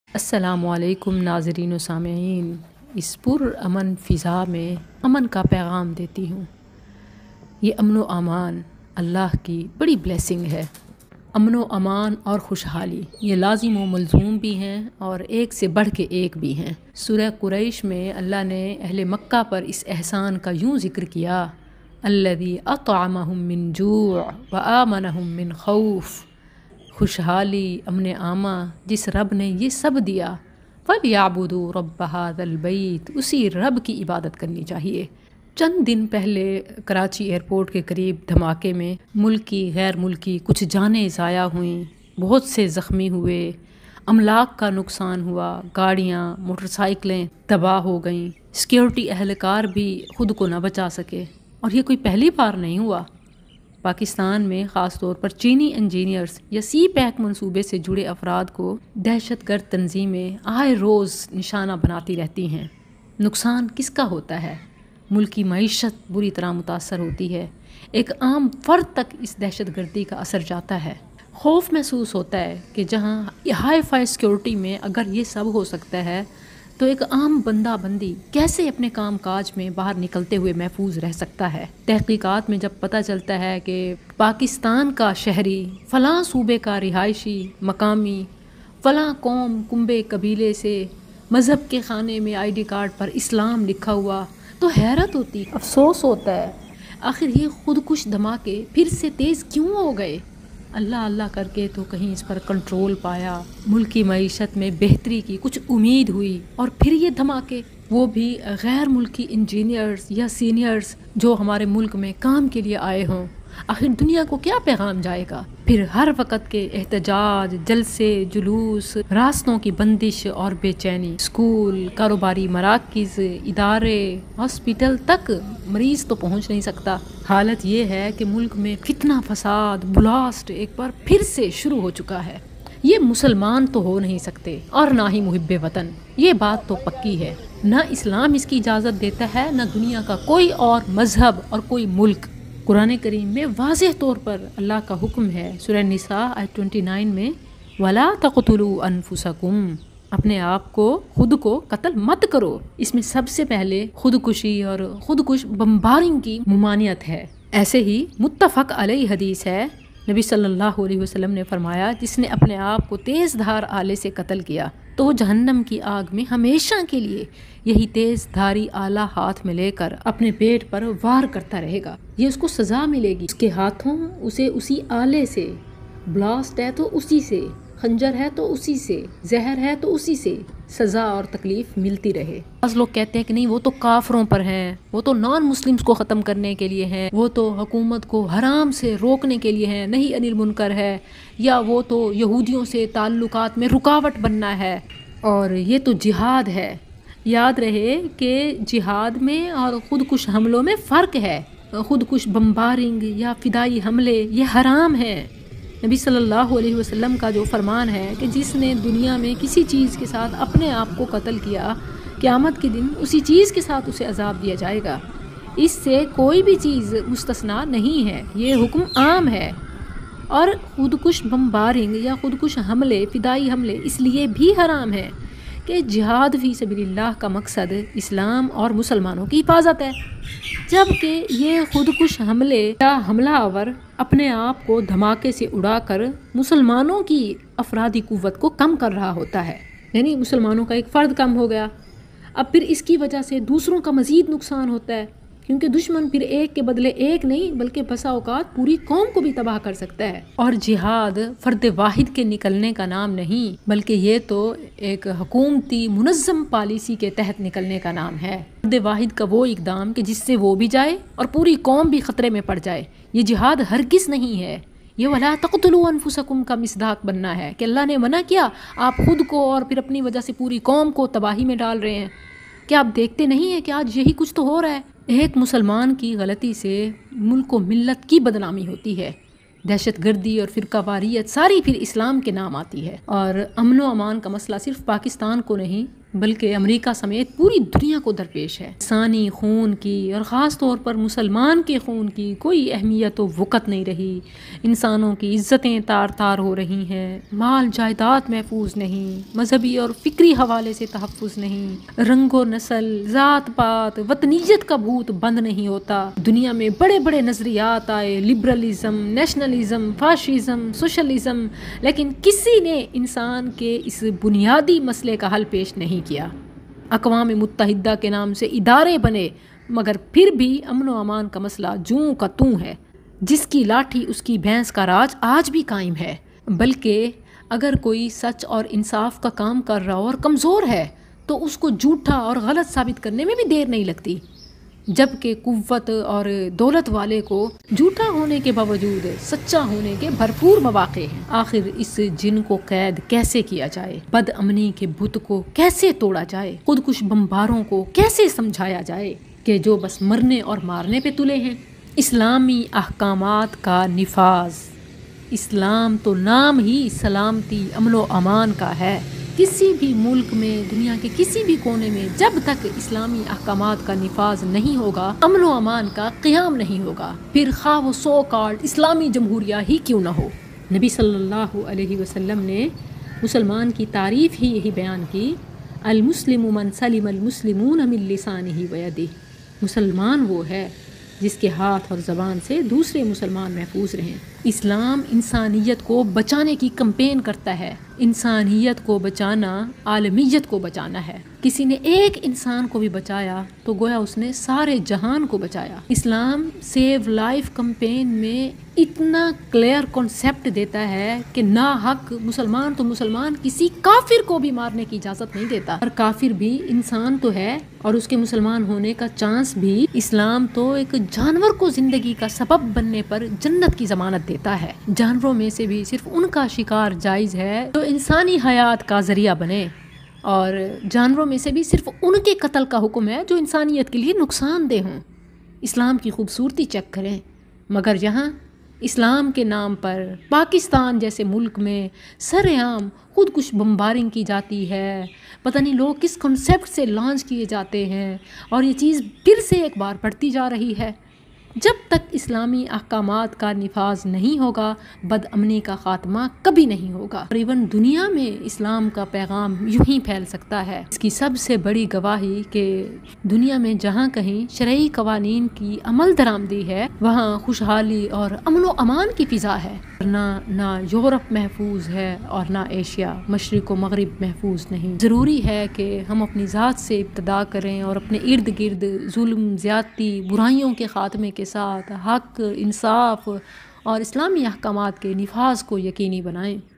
Chaini logo par khudkush hamla | Dr Misbah Ahmed
چینیوں پر حملے آخر مقاصد کیا ہیں؟ | سیکیورٹی ادارے کیا کر رہے ہیں؟ | پر امن فضاؤں سے امن کا پیغام | ڈاکٹر مصباح احمد

Follow Dr Misbah ahmad